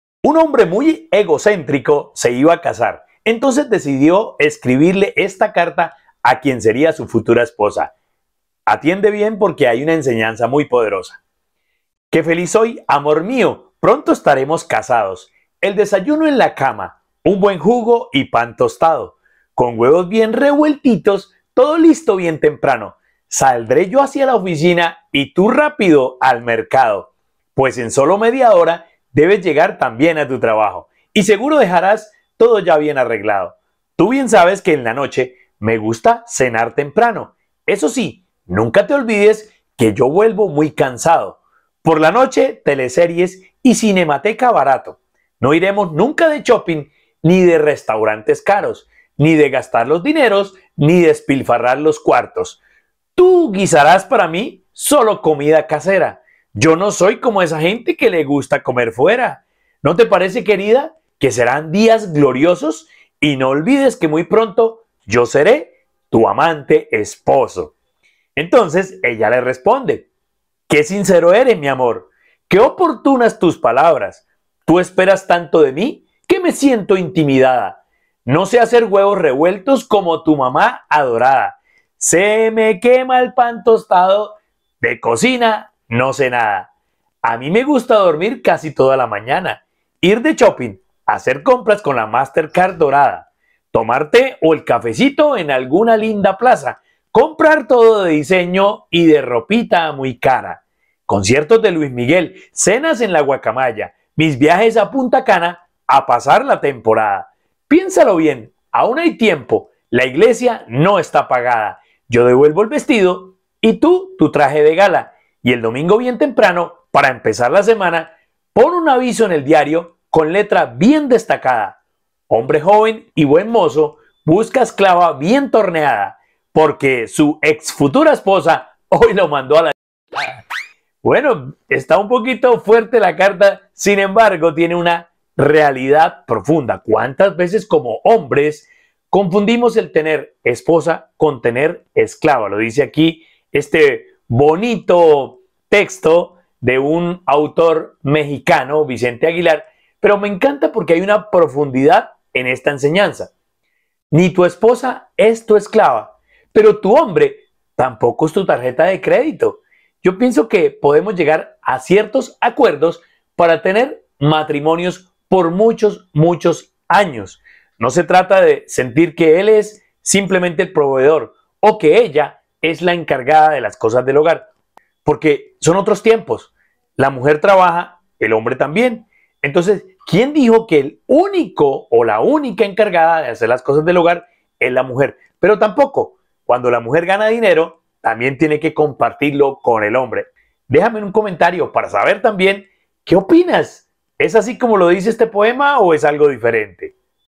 Carta de un hombre antes de casarse - Reflexiones de Familia
Este poema te llevará a analizar que el mundo cambió totalmente, en cuanto a la forma como las personas llevan su vida en pareja, el mundo ha cambiado y muchas veces seguimos viviendo en los hábitos pasados.

Crecimiento personal
Carta de un hombre antes de casarse
La forma de llevar la relación de pareja en esta época.